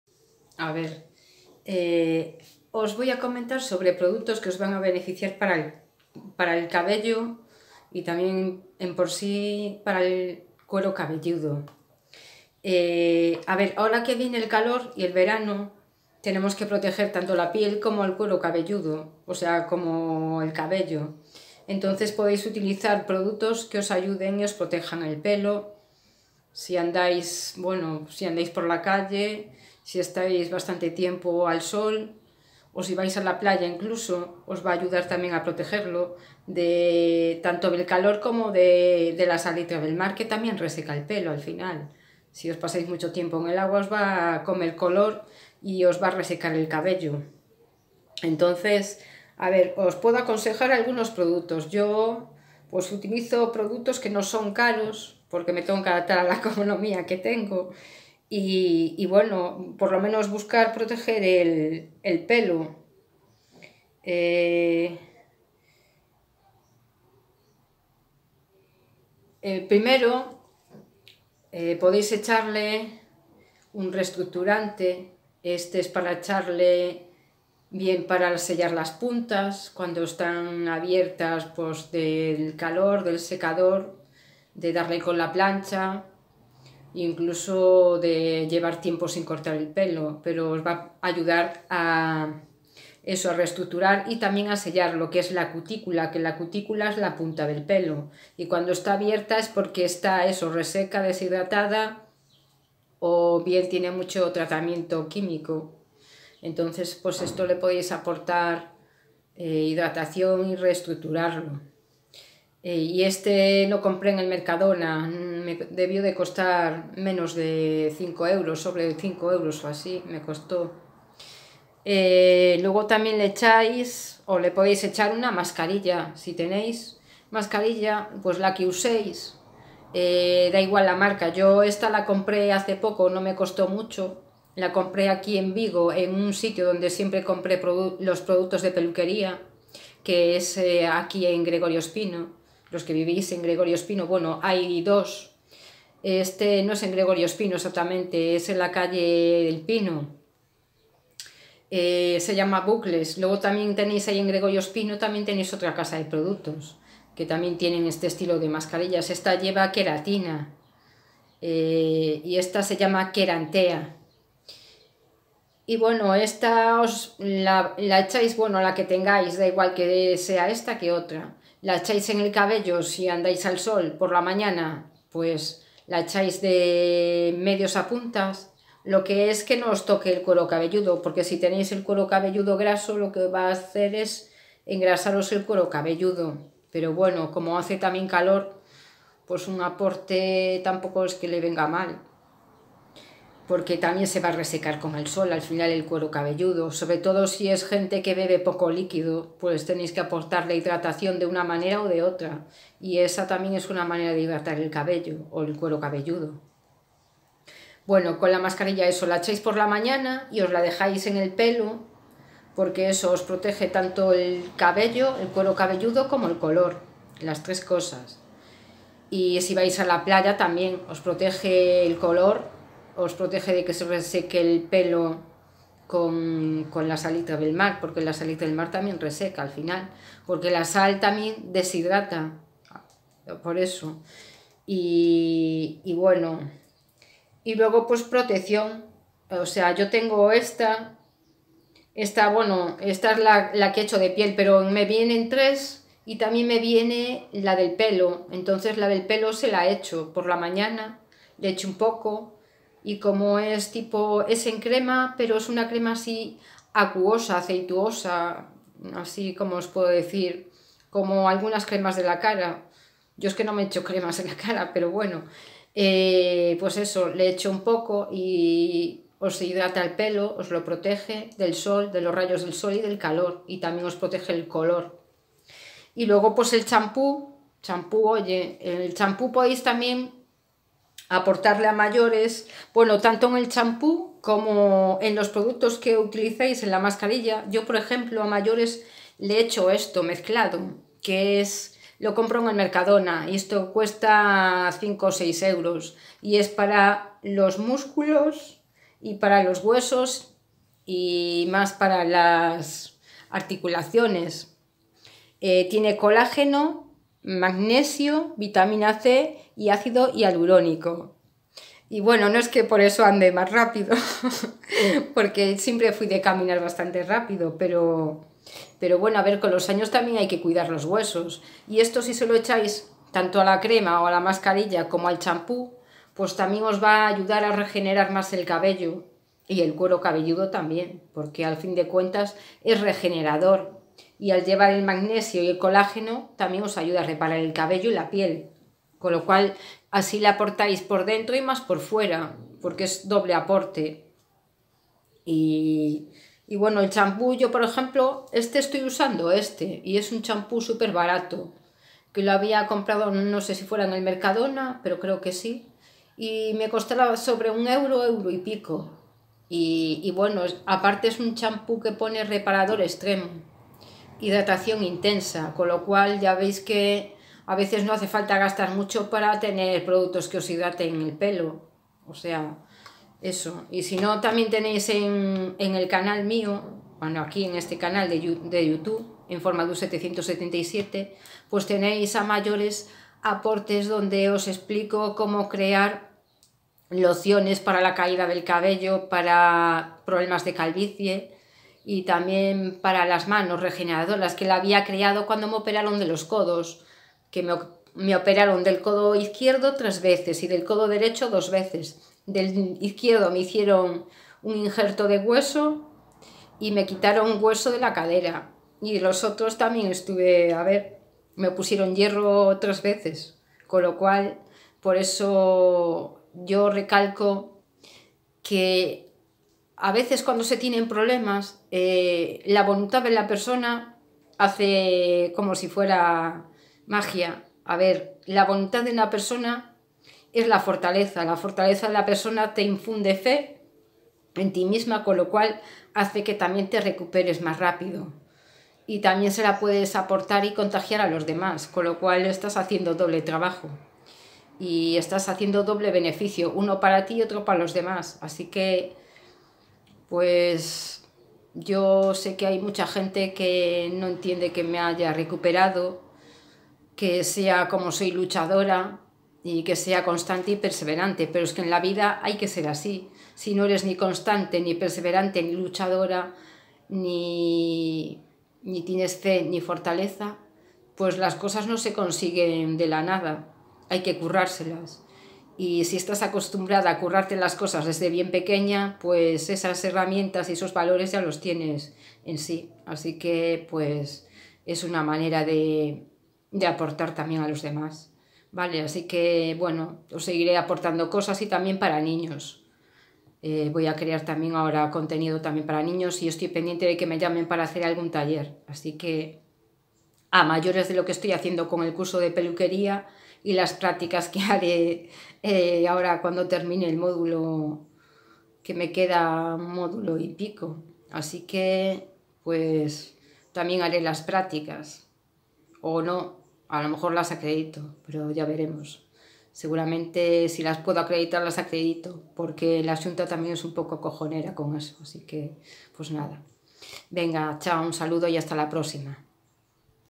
consejos y productos para el cuidado del cabello y la piel
este es un vídeo hecho para dar algún consejo sobre el cuidado del cabello y de la piel. Productos y alguna cosa más que os puede ayudar.